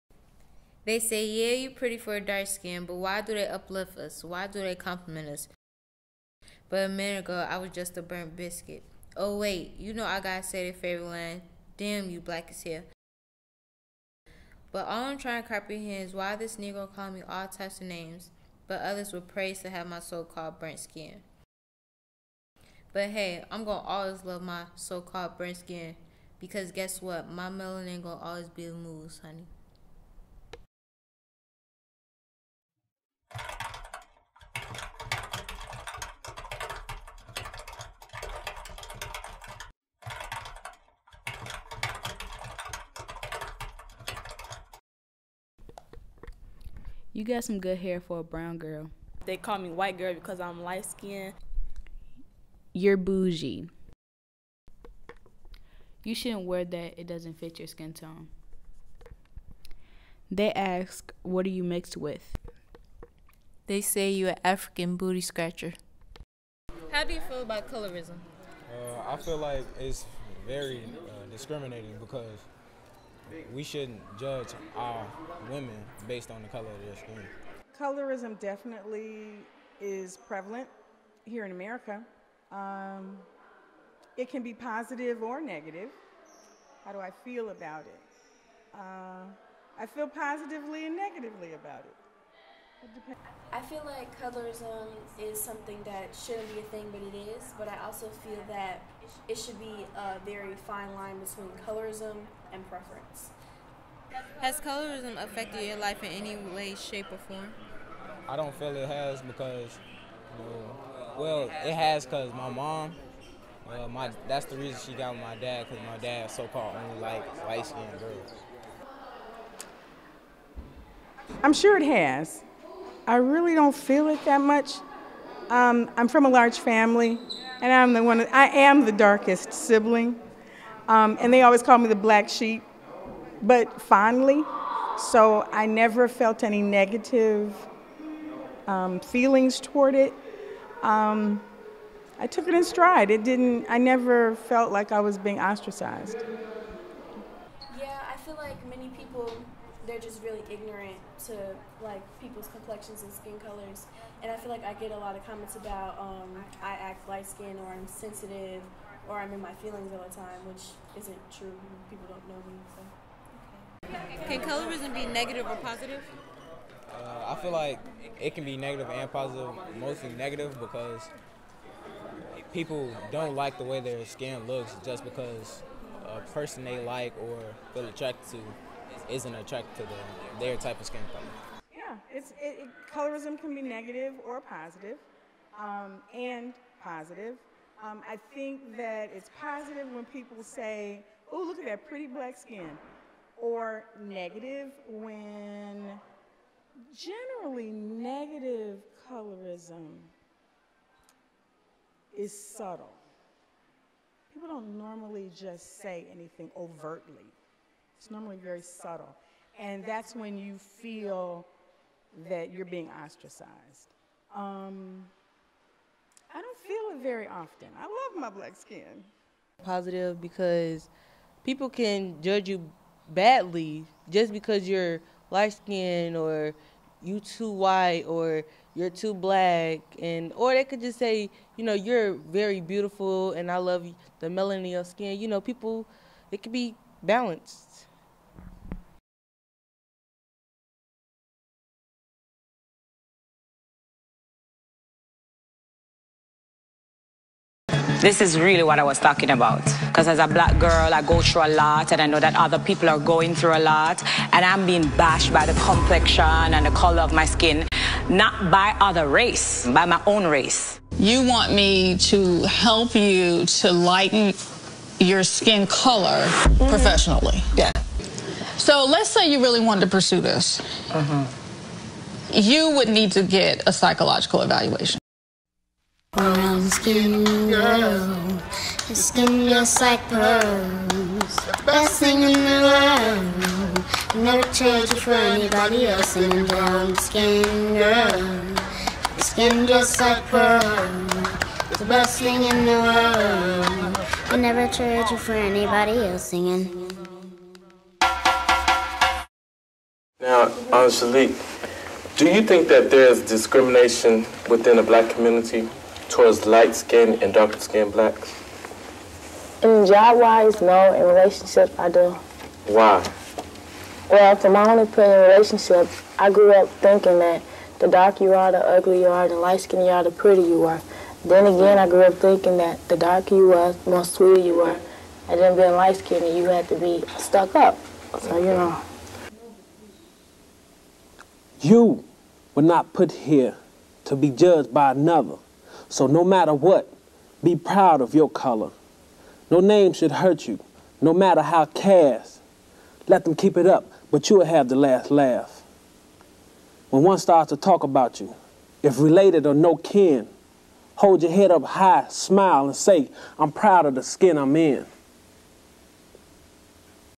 How Do You Feel About Colorism? - South Shore High School (2019)
Smartphones makes the world available in everyone's pocket. Unfortunately, it is also a bullying tool in the palm of everyone's hand. In, "How Do You Feel About Colorism?" South Shore students learn how to address this form of bullying today, compared to the 1990s.

This video is part of a series in partnership with Yvonne Welbon and her nonprofit, Sisters In Cinema, which works to entertain, educate, develop and celebrate Black girls and women media-makers, as well as future generations of female storytellers.
This series is also made possible by the Chicago Community Trust!